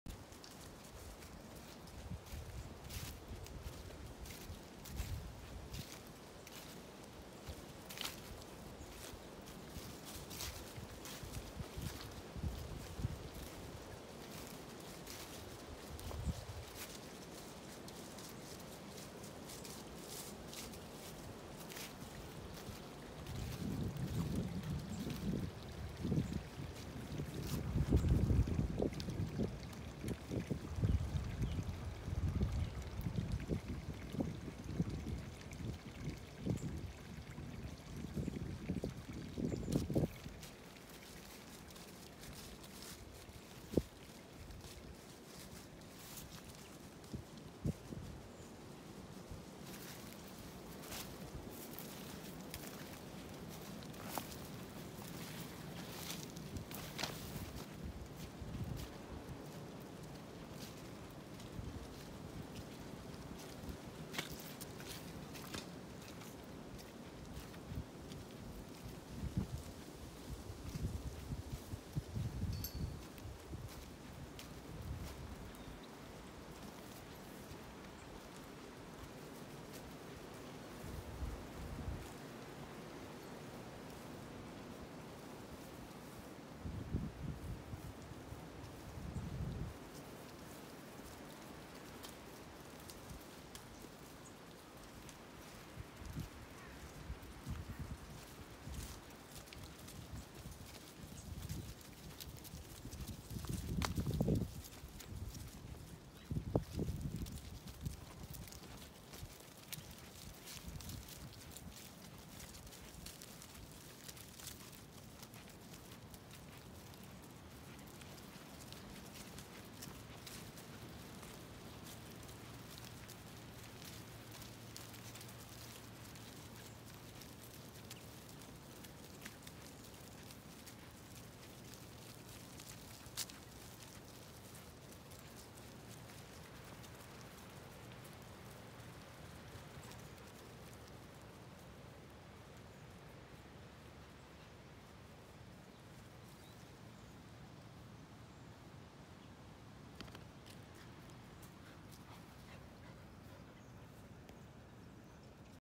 Come for a December Walk with Me
Come for a walk with me amongst the Zwartbles sheep across a corner of a field to see where I have placed our ewes hay. December, winter farm life.